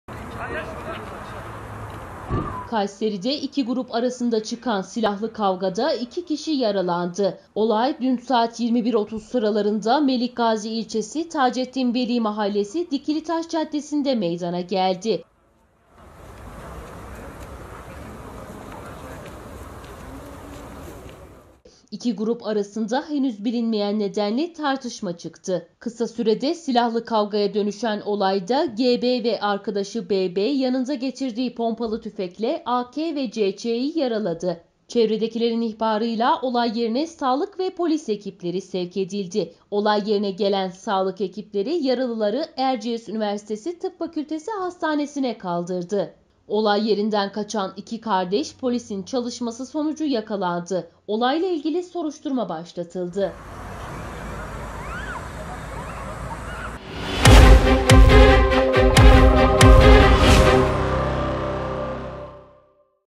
Kayseri'de iki grup arasında silahlı kavga: 2 yaralı
KAYSERİ'de, iki grup arasında çıkan silahlı kavgada 2 kişi yaralandı.

Olay, saat 21.30 sıralarında Melikgazi ilçesi Tacettin Veli Mahallesi Dikilitaş Caddesi'nde meydana geldi. İki grup arasında henüz bilinmeyen nedenle tartışma çıktı. Kısa sürede silahlı kavgaya dönüşen olayda, G.B. ve kardeşi B.B. yanında getirdiği pompalı tüfekle A.K. ve C.Ç.'yi yaraladı. Çevredekilerin ihbarıyla olay yerine sağlık ve polis ekipleri sevk edildi. Olay yerine gelen sağlık ekipleri, yaralıları Erciyes Üniversitesi Tıp Fakültesi Hastanesi'ne kaldırdı. Olay yerinden kaçan iki kardeş ise polisin çalışması sonucu yakalandı.